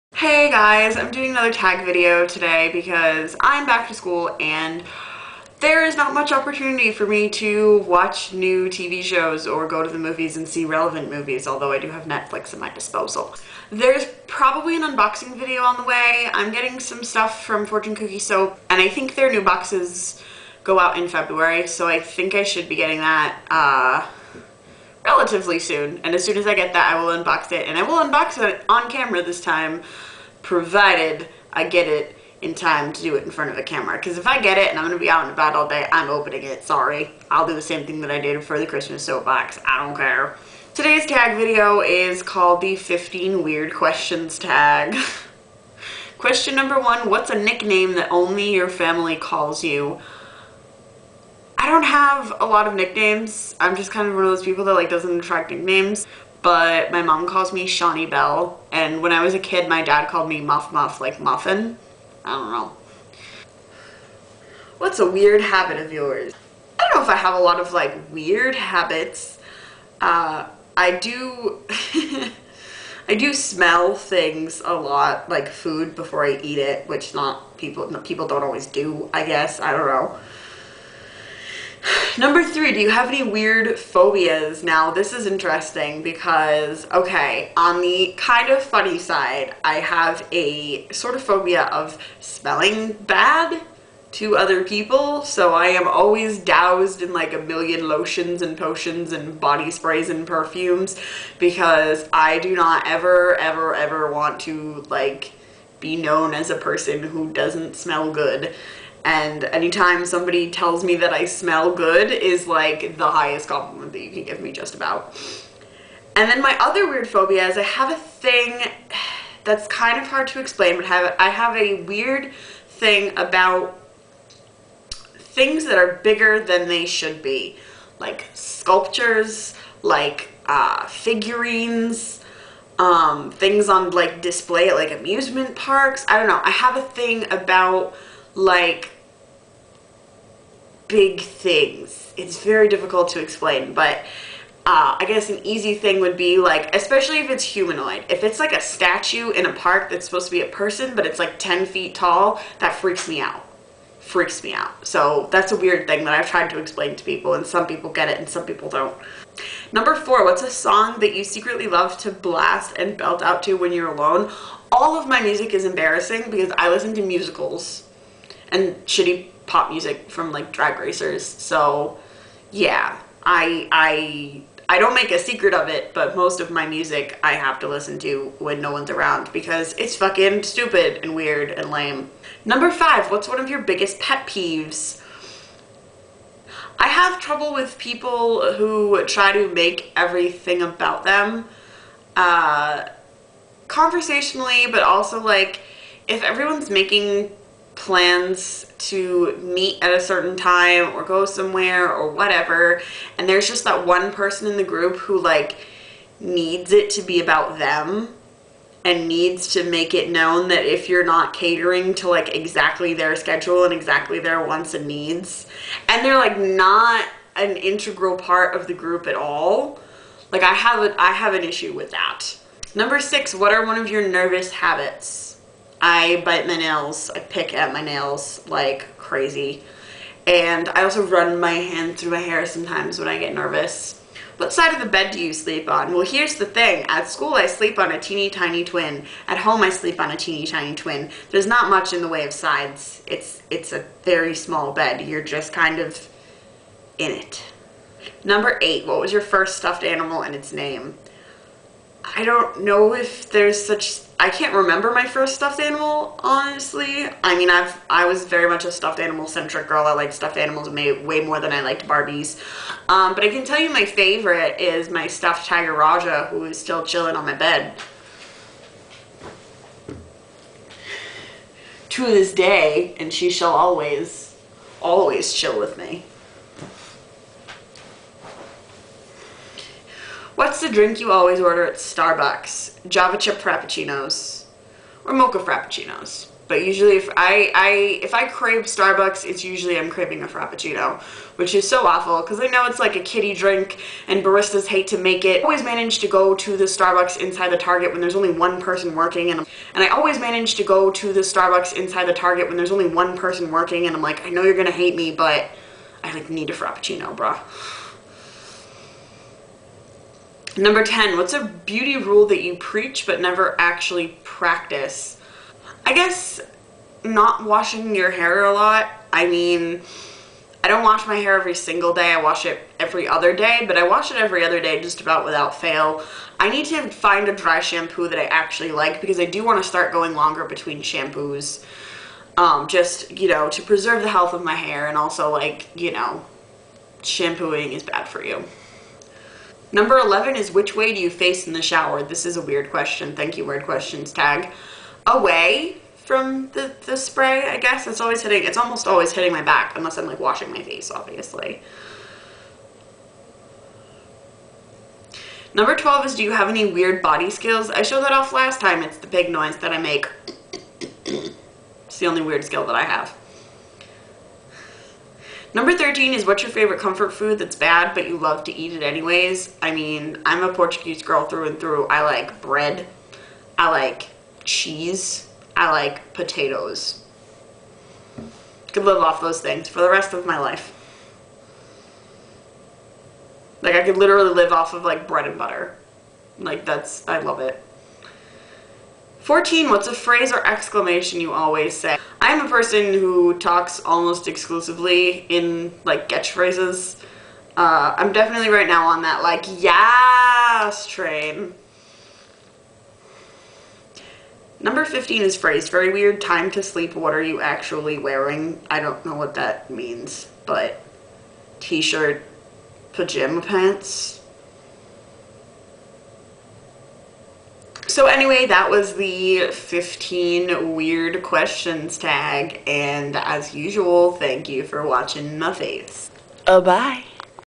A Weird Questions Tag
WEIRD QUESTIONS
15 Weird Questions Tag

The Questions:

#1. What's a nickname only your family calls you?
#2. What's a weird habit of yours?
#3. Do you have any weird phobias?
#4. What's a song you secretly love to blast and belt out when you're alone?
#5. What's one of your biggest pet peeves?
#6. What's one of your nervous habits?
#7. What side of the bed do you sleep on?
#8. What was your first stuffed animal and it's name?
#9. What's the drink you always order at starbucks?
#10. What's a beauty rule you preach, but never actually practice?
#11. Which way do you face in the shower?
#12. Do you have any 'weird' body 'skills'?
#13. What's your favorite comfort food thats 'bad' but you love to eat it anyways?
#14. What's a phrase or exclamation you always say?
#15. Time to sleep, what are you actually wearing?